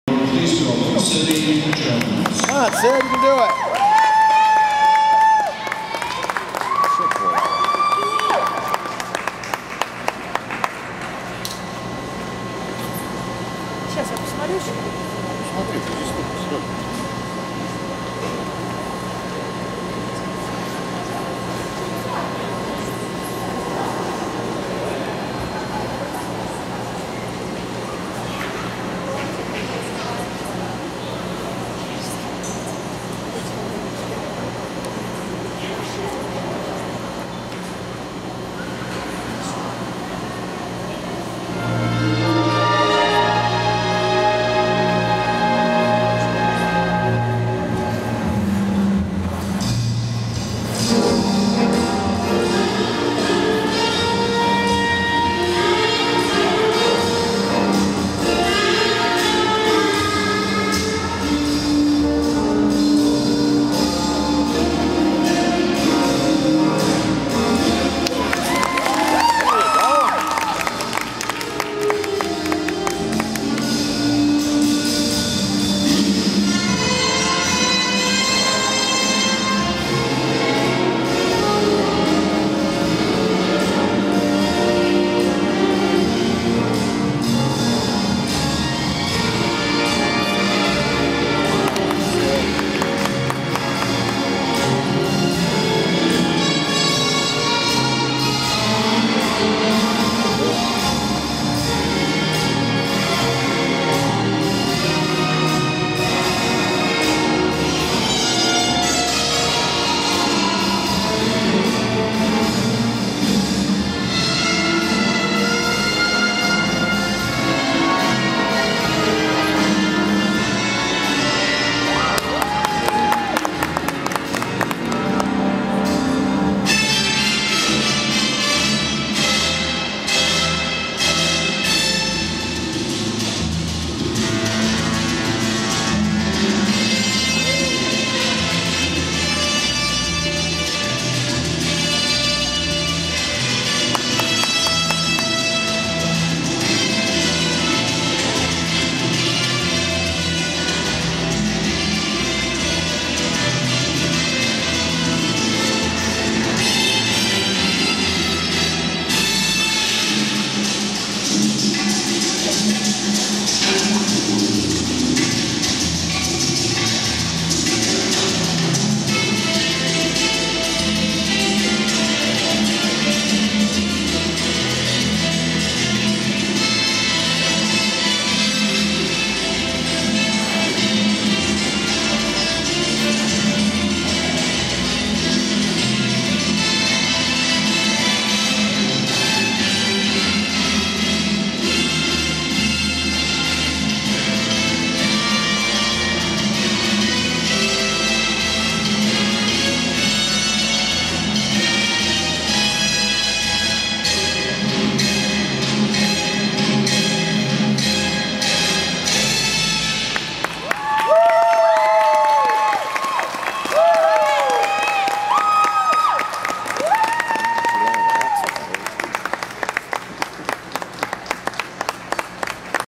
Sydney Jones - Junior Ladies Short Program, Lake Placid, 2011
Sydney placed 4th out of 17 with a personal best in points, 38.11, just 1.2 off from first place.